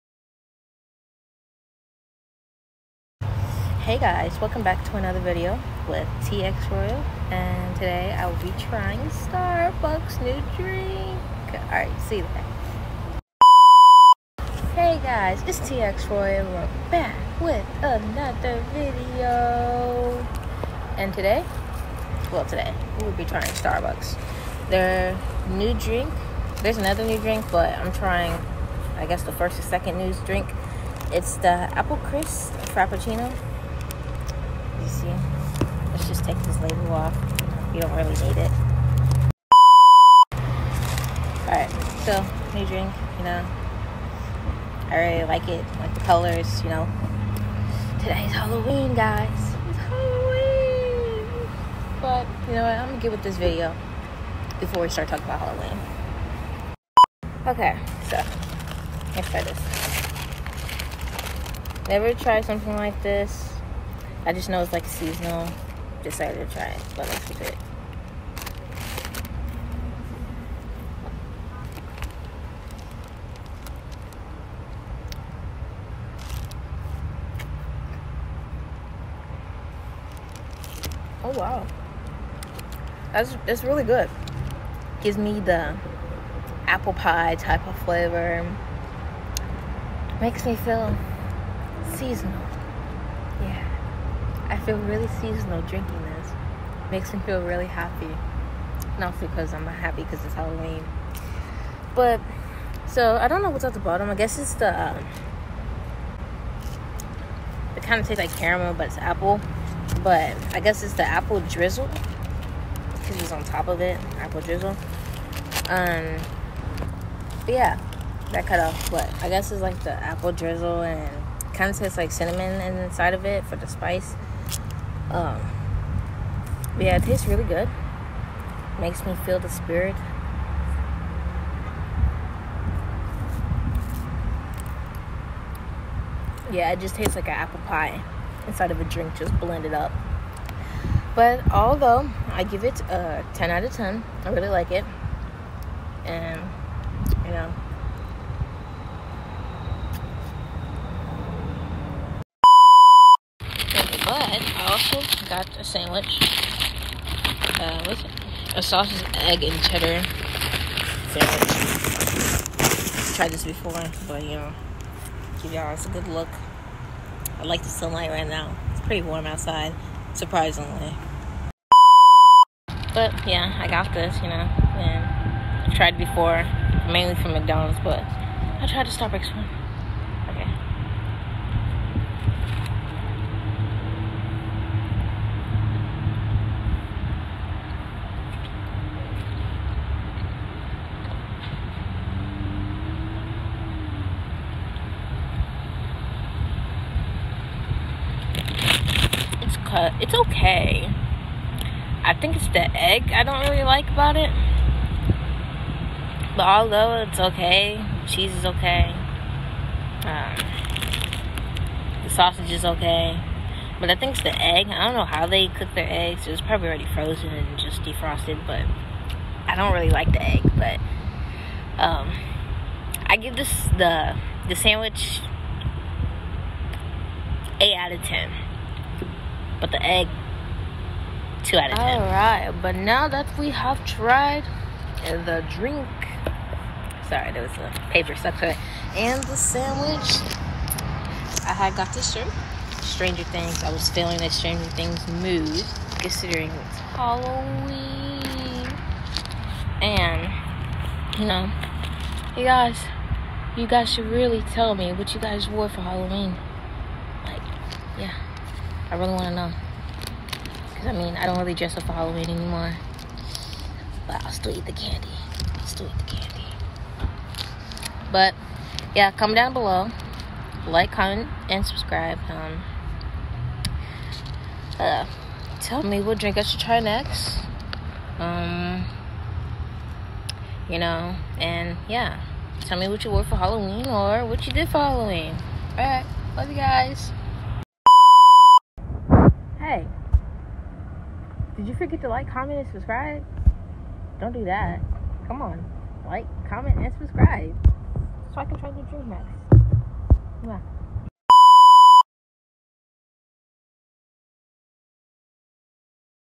Starbucks - trying the new Apple Crisp Frappe￼ drink
Welcome Royal Munchers, I enjoyed trying this new Starbucks Apple Crisp Frappe coffee and tasty sandwich. Watch for my reaction and food review.

😁Royal out ✌🏽